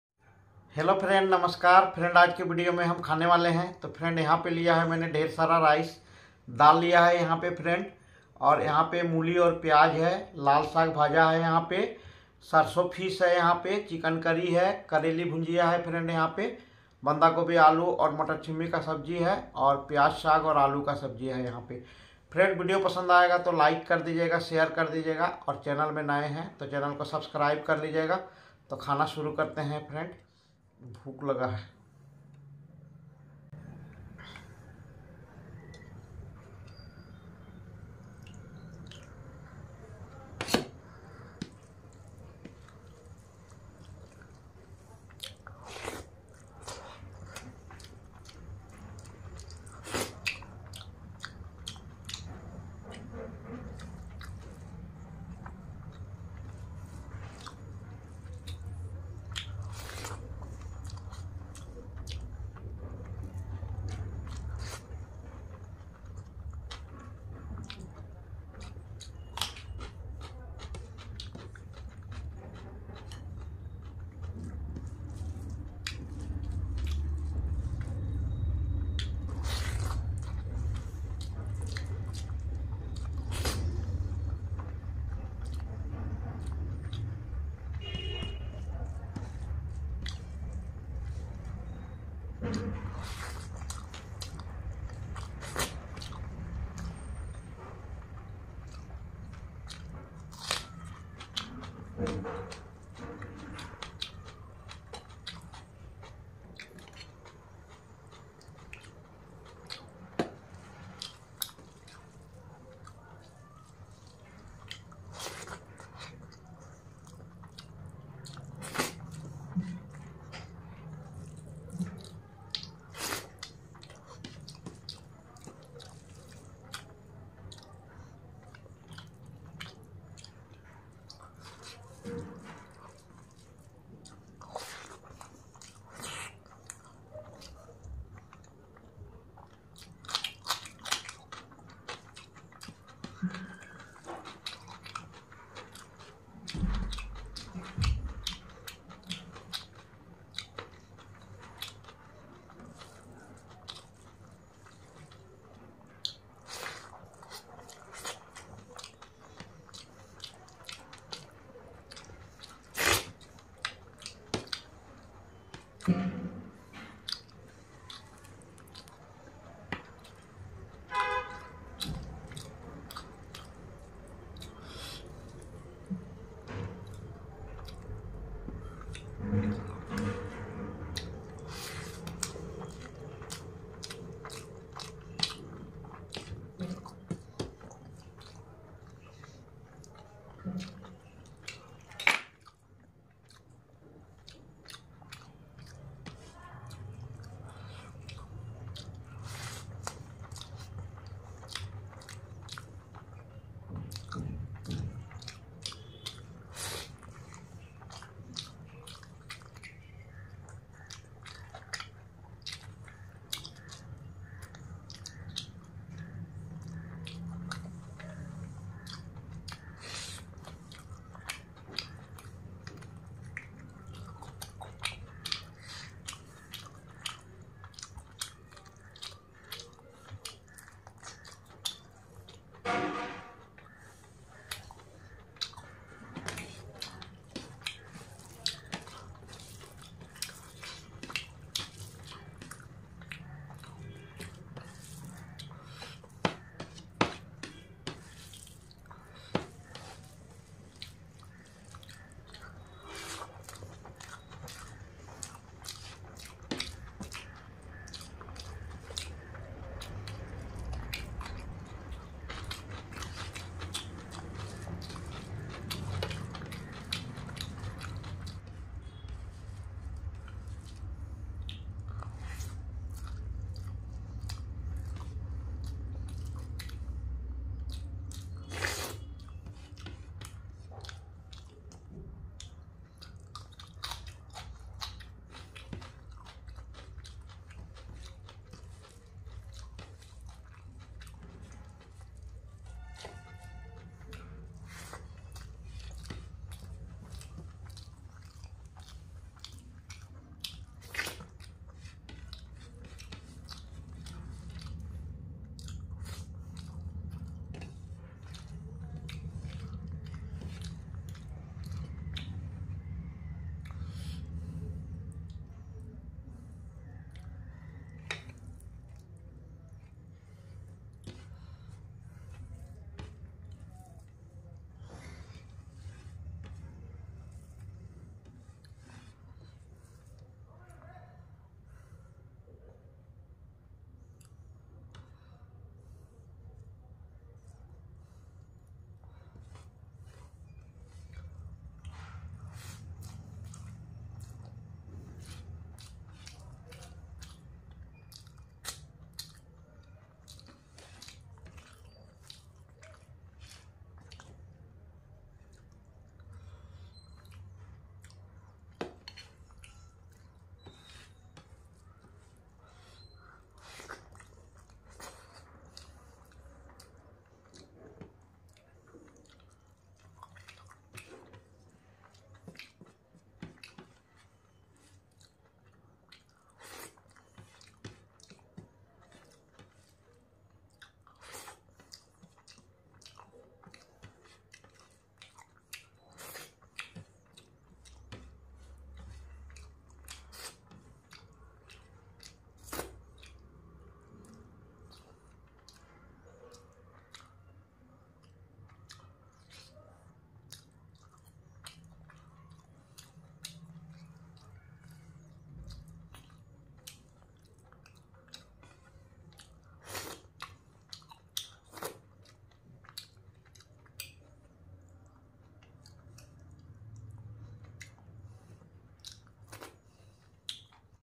ASMR EATING CHICKEN CURRY RICE 😋| BANGALI EATING SHOW | INDIAN LUNCH EATING | MUKBANG EATING SHOW
HELLO GUYS IN THIS VIDEO I WILL SHOW YOU A SIMPLE  EATING SHOW. IT WILL BE LUNCH EATING. ITS AN NONVEG EATING SHOW AND BENGALI FOOD EATING
Eating show | Bangali eating show | Mukbang | Eating challenge | ASMR | Eating show Indian